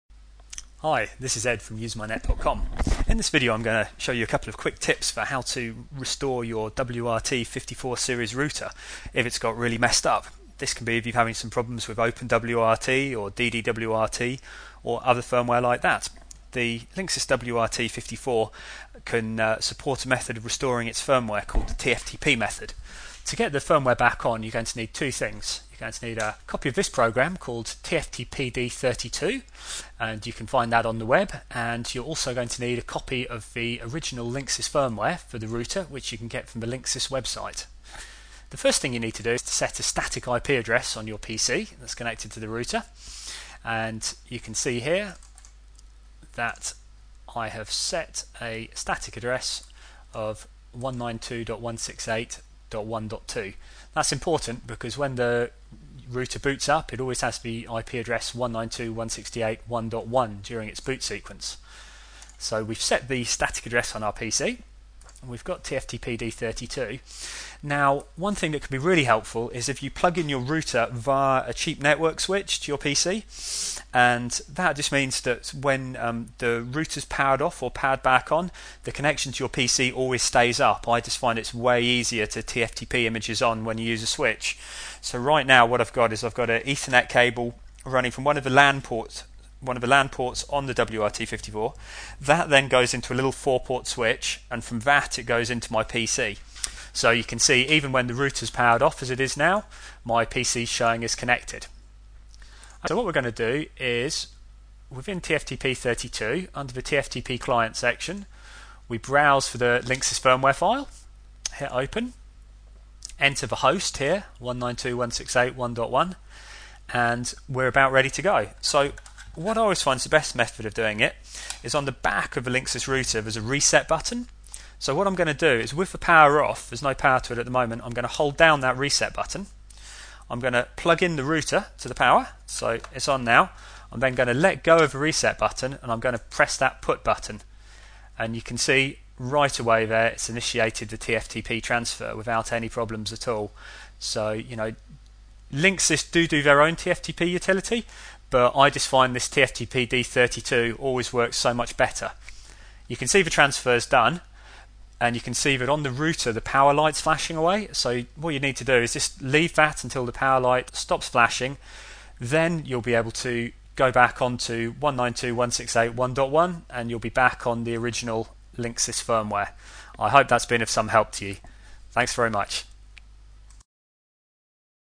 Restore Firmware on WRT-54 series - UseMyNet.com
- This demo shows you how to TFTP the original firmware on the WRT-54 series routers very simply.. Also for more information on Hotspot Wifi Routers and Software